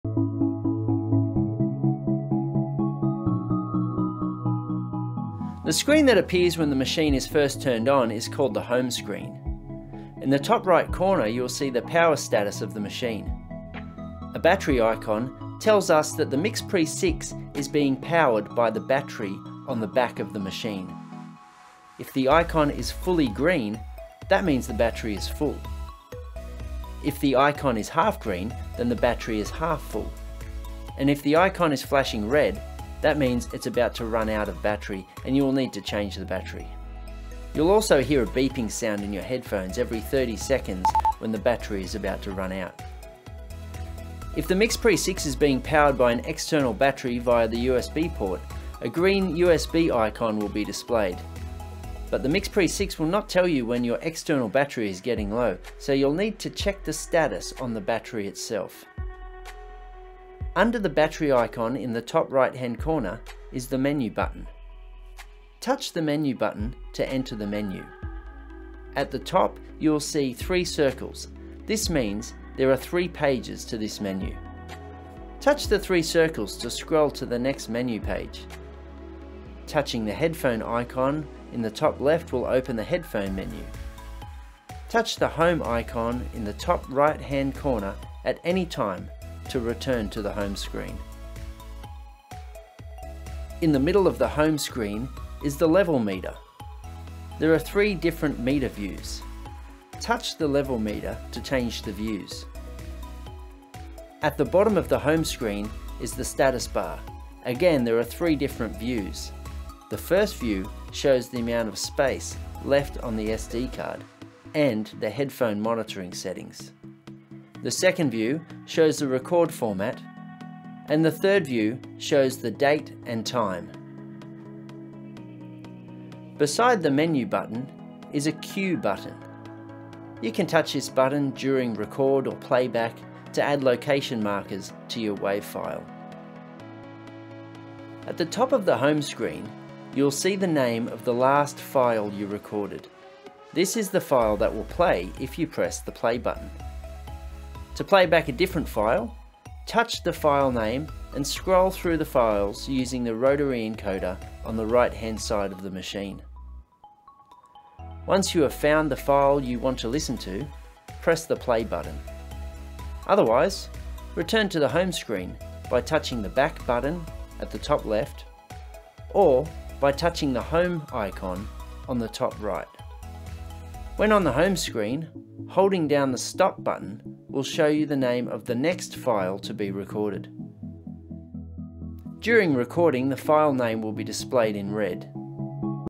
MixPre Series Episode 05 - The Home Screen
Making a great recording starts with using quality gear. This video series will get you set up with the Sound Devices MixPre6 and run through all the settings to make sure you are recording in the best format. The series will cover loading pre-sets, creating new projects, channel settings, level control and monitoring, powering options, date and time settings and data storage. Although this series is showing the MixPre6, much of the information is also relevant for users of the MixPre3 and MixPre10.